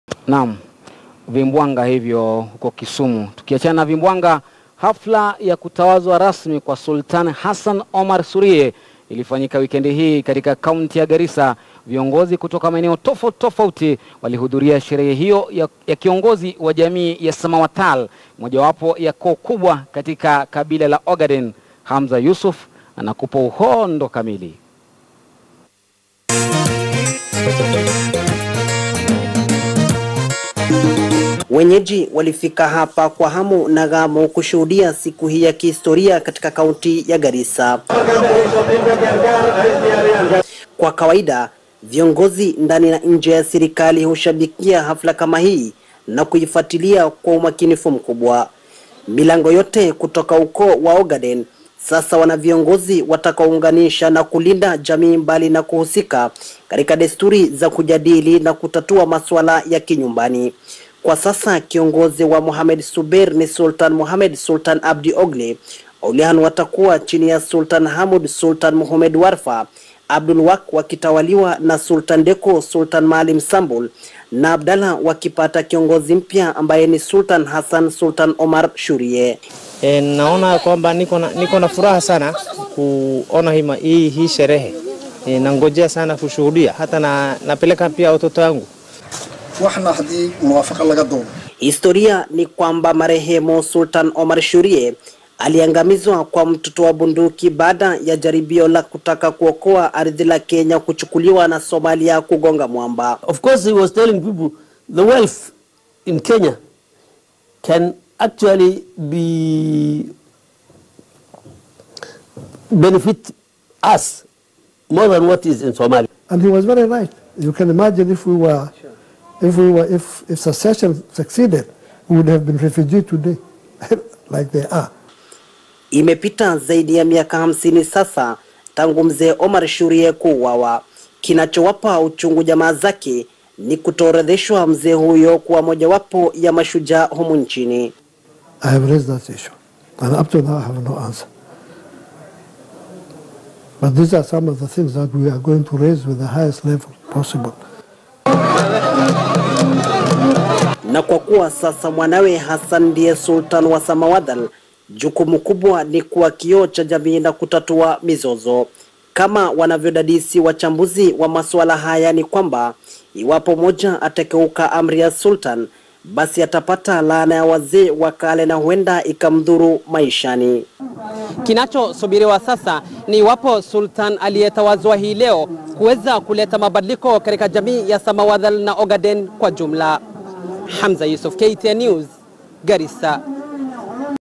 Sherehe ya Kumsimika mfalme Sultan Hassan Omar Shuriye
Hafla ya kutawazwa rasmi kwa Sultan Hassan Omar Shuriye ilifanyika wikendi hii katika kaunti ya Garissa. Viongozi kutoka maeneo tofauti walihudhuria sherehe hiyo ya kiongozi wa jamii ya Samawathal  mojawapo ya koo kubwa katika kabila la Ogaden. Hamza Yussuf anakupa taarifa hiyo kwa kina.